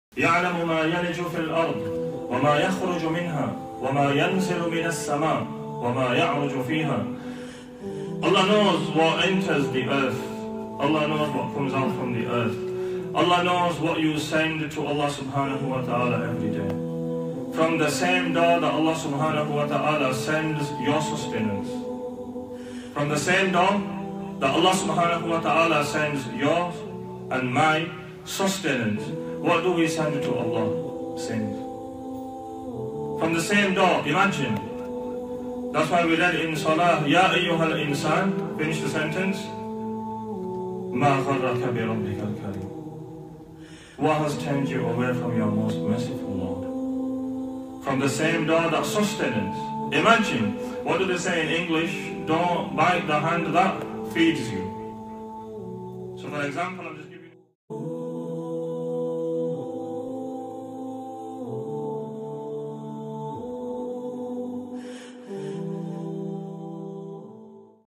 What have you sent to Allah swt today shaikh Khalid Sharief
Short Islamic reminders